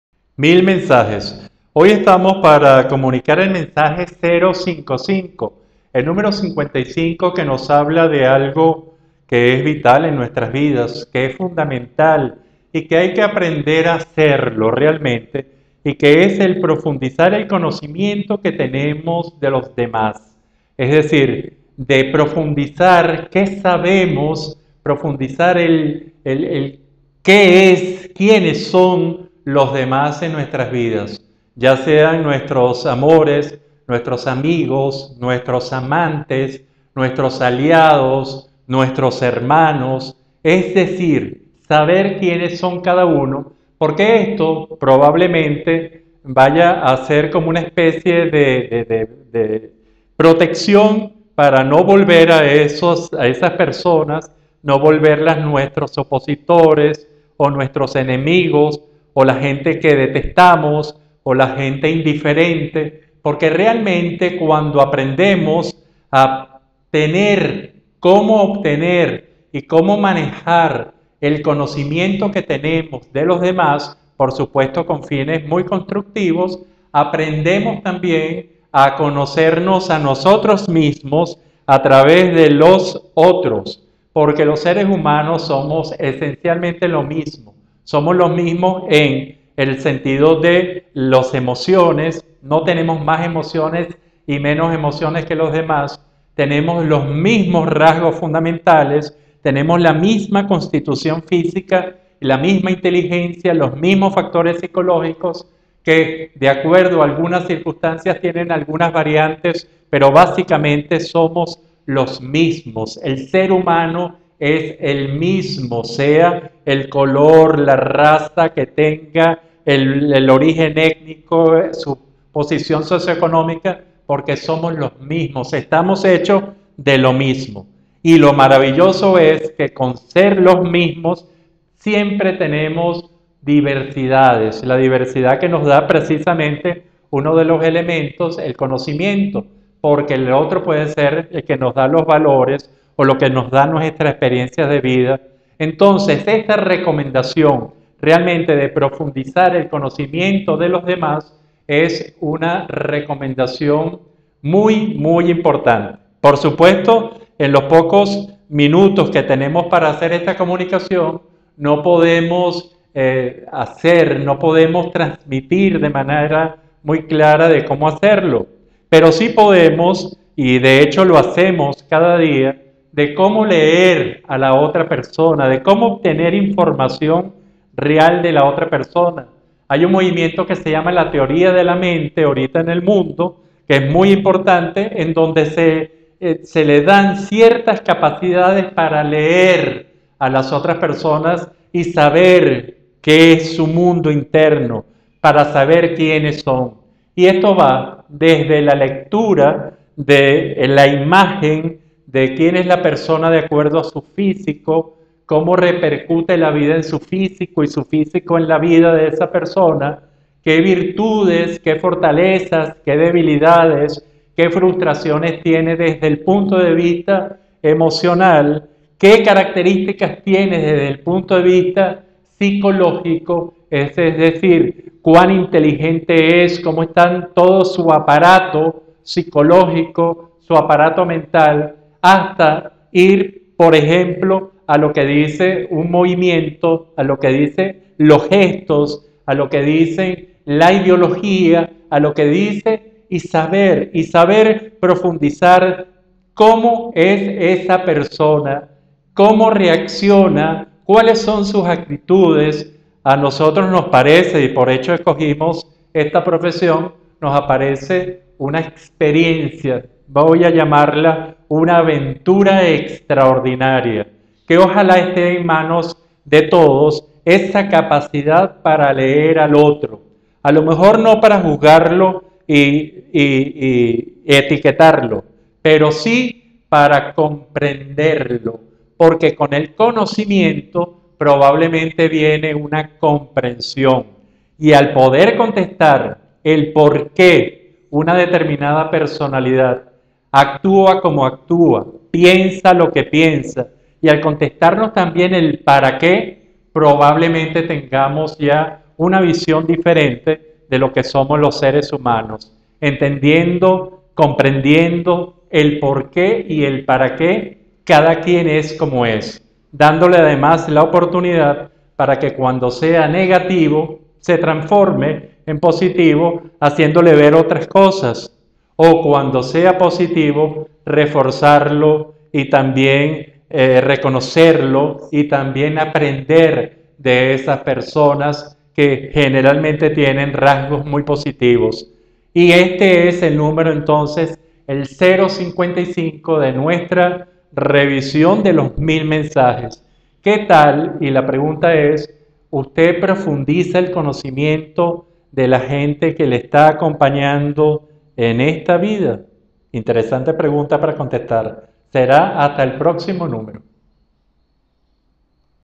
055 Mil Mensajes Profundizar el Conocimiento de los Otros
Reponiendo los Mensajes borrados.

La profundización del conocimiento que se tiene de los otros seres humanos que nos rodean, que nos acompañan en los más diferentes roles en las más diversas áreas de nuestras vidas, es necesaria y, por lo tanto, hay que entrenarse para hacerlo en la forma más eficaz posible.